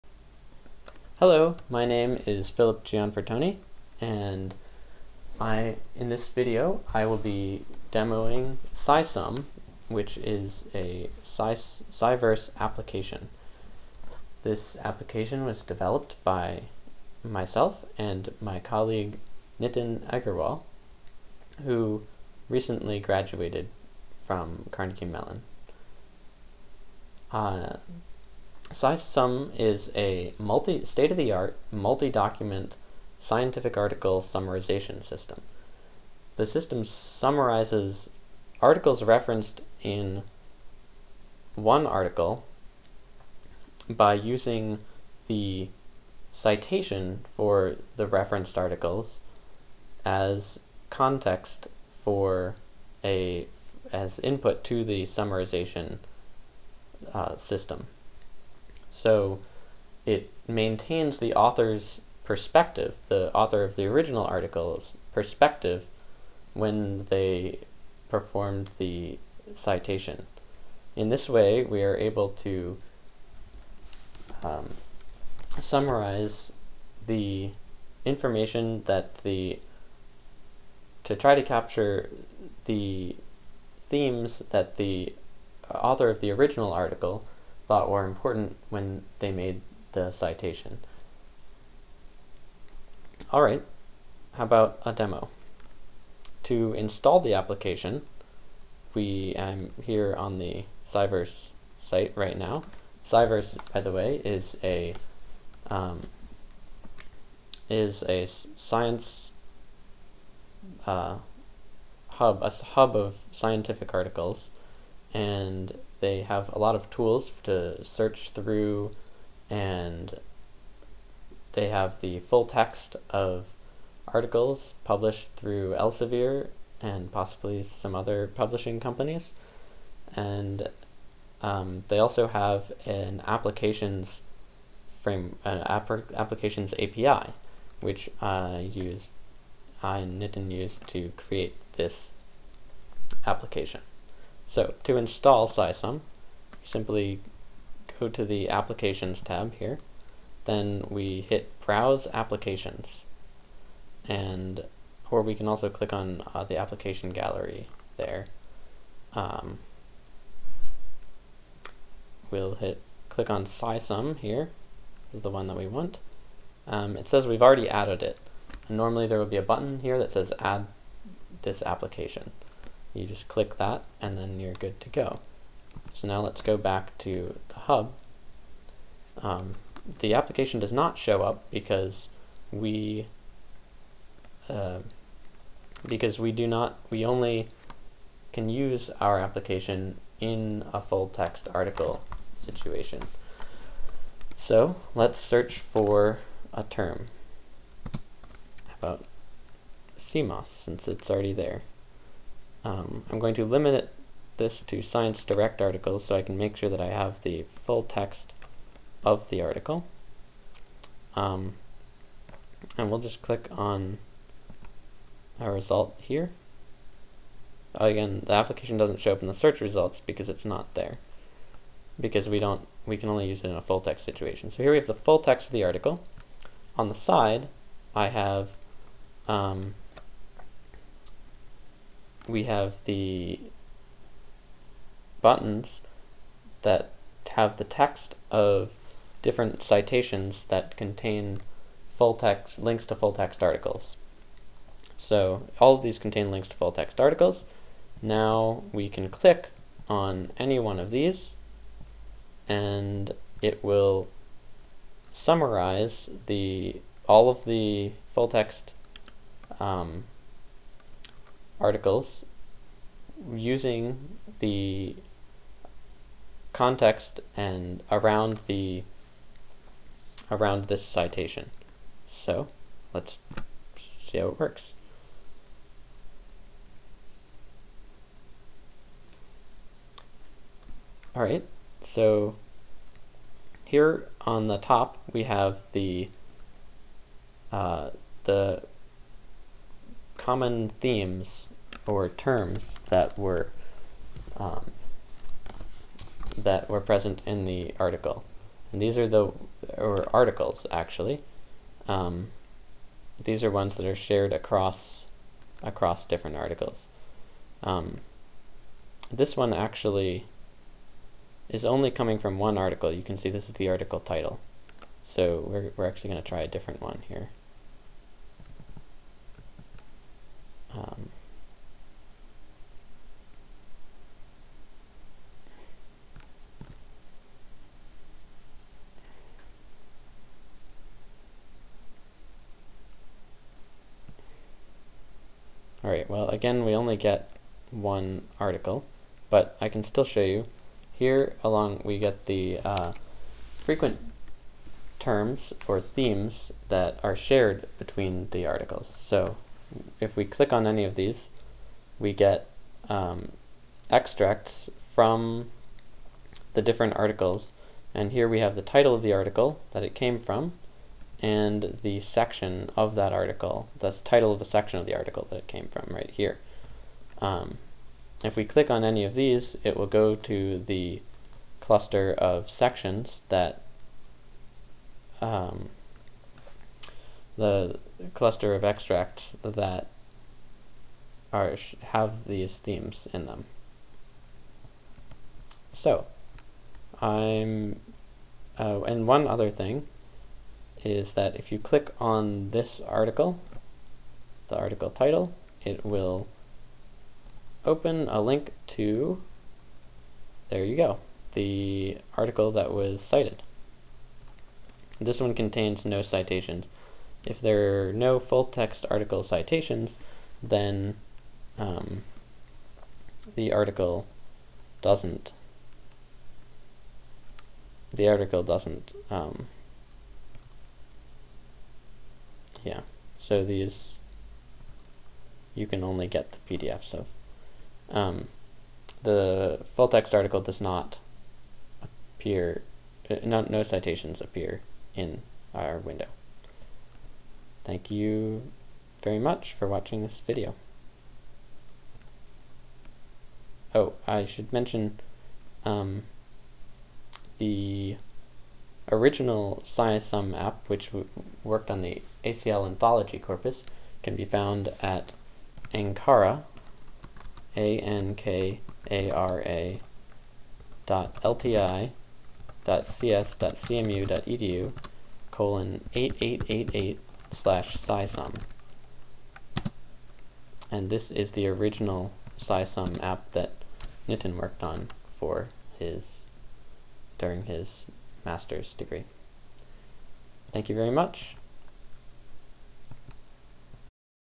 SciSumm Demo
SciSumm is a state-of-the-art scientific article summarization system.  It summarizes the reference articles cited in a full text article when available using the citation and surrounding text to make the summary relevant.  In this way, the original author's perspective is used to help guide the summarization process.

for a demo using the ACL 2008 corpus.

The is a demo of the version created for the Apps for Science Challenge 2011

The original system was developed by Nitin Agarwal at Carnegie Mellon University.  This Sciverse interface was developed by Nitin Agarwal and Philip Gianfortoni under Professor Carolyn Penstein Rosé.

The original system was implemented in Java and JSP using the Spring framework.  The new interface was implemented in javascript using the jquery library.